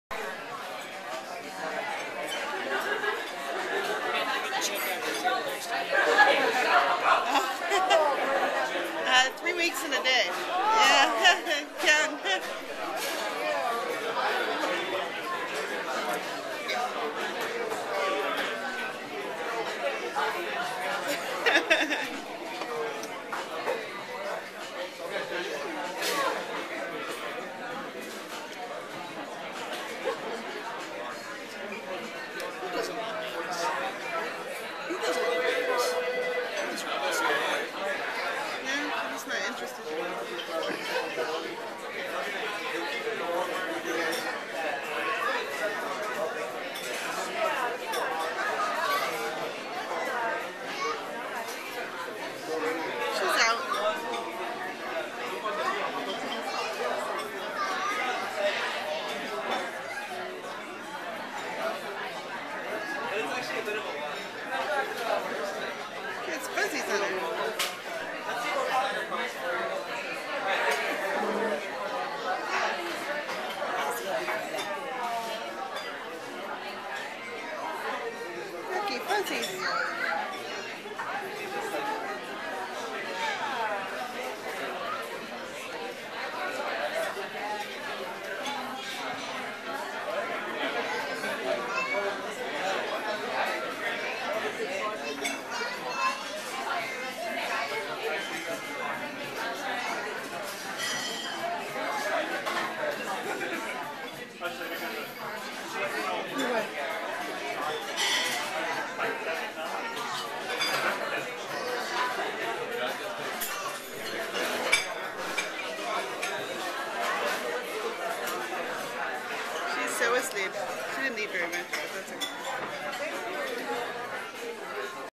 Megan Agi Biro "eats" Chicago Style Pizza
We took Megan to Patxi's Pizza in Palo Alto where we had Chicago Style Pizzas (and some $1 PBRs) and she had a bottle of formula